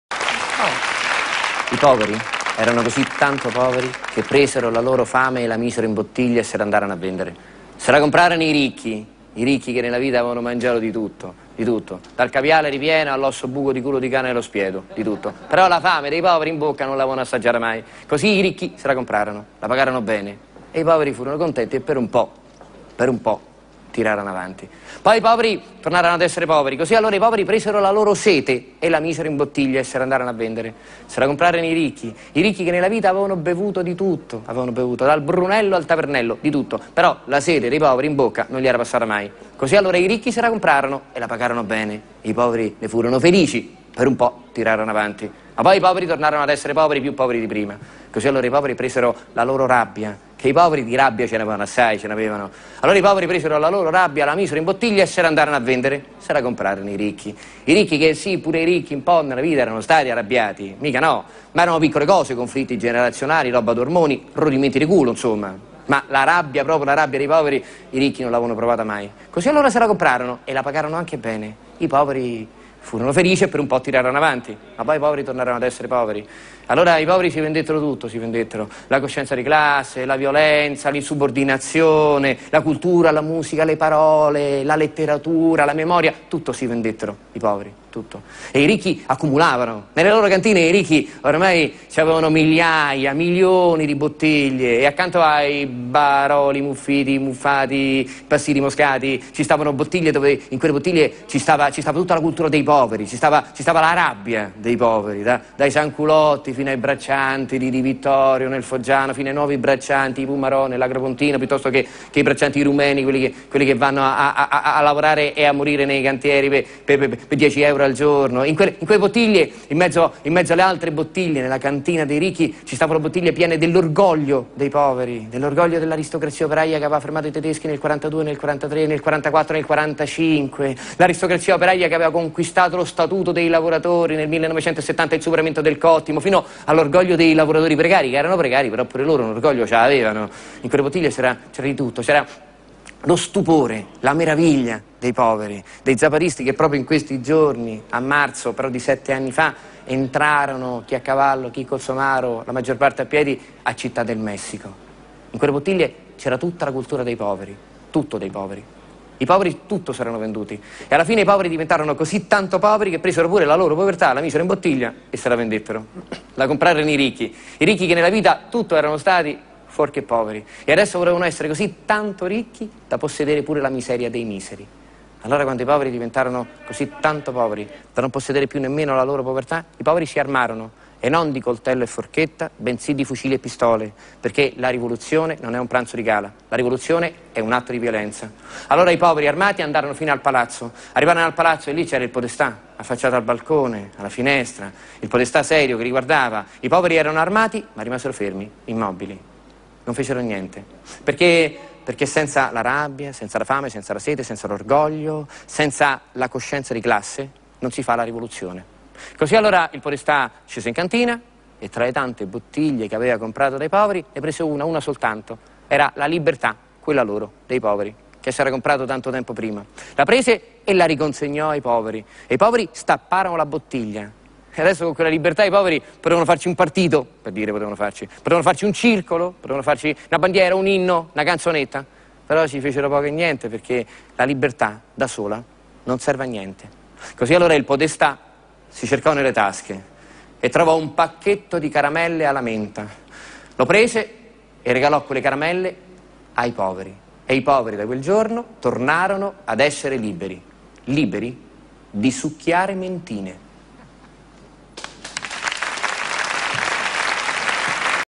La libertad de los pobres [subtitulado] - Ascanio Celestini
Intervención de Ascanio Celestini, actor y dramaturgo italiano, que narra una fábula acerca de la libertad, la de los pobres y de cómo sin la rabia, sin el hambre, sin la sed, sin el orgullo, sin la conciencia de clase, no se hace una revolución, la revolución es pues, un acto de violencia.